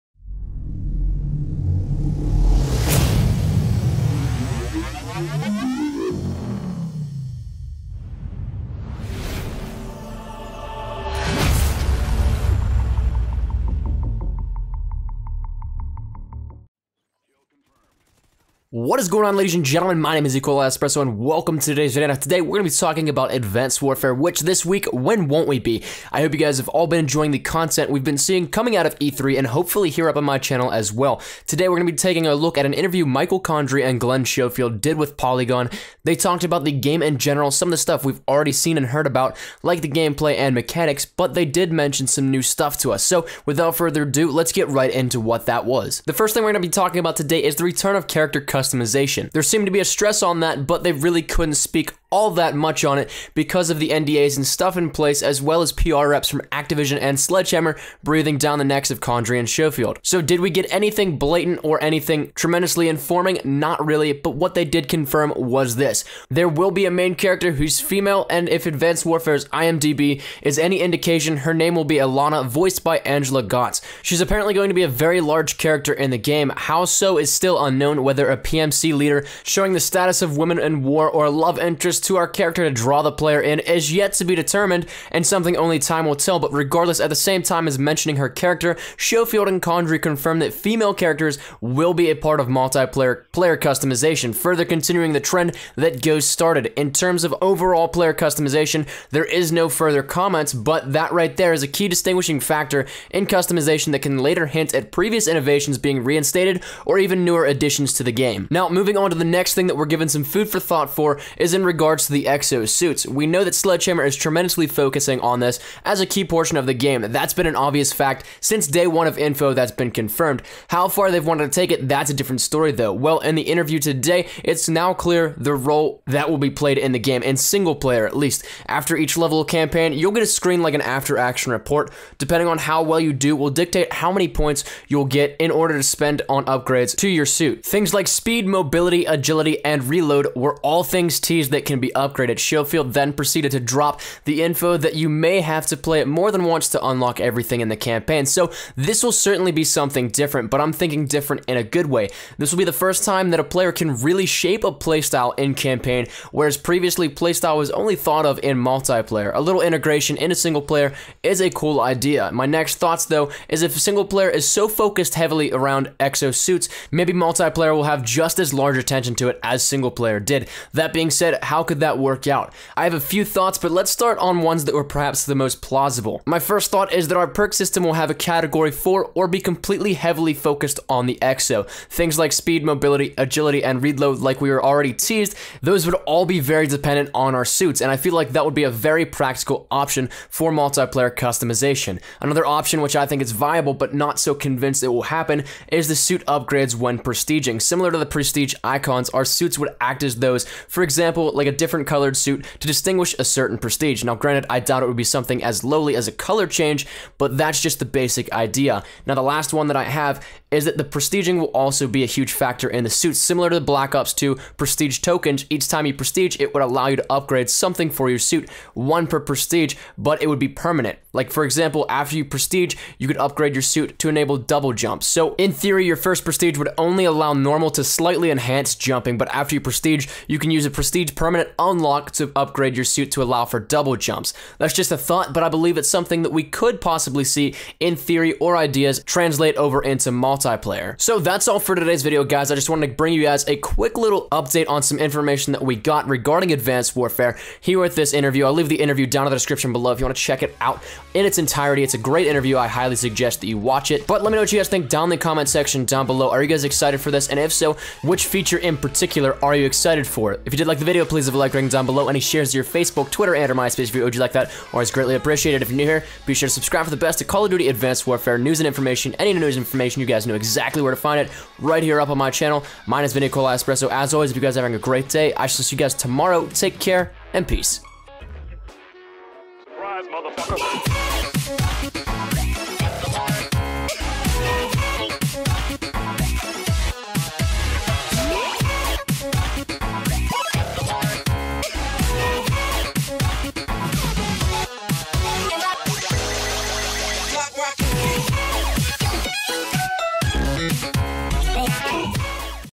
Call of Duty: Advanced Warfare - EXO SUIT UPGRADE SYSTEM - FEMALE CHARACTERS IN SINGLE/MULTIPLAYER
What is going on ladies and gents!? Today we're going to be talking about the new information that we have just received regarding Advanced Warfare and the exo suits as well as character customization.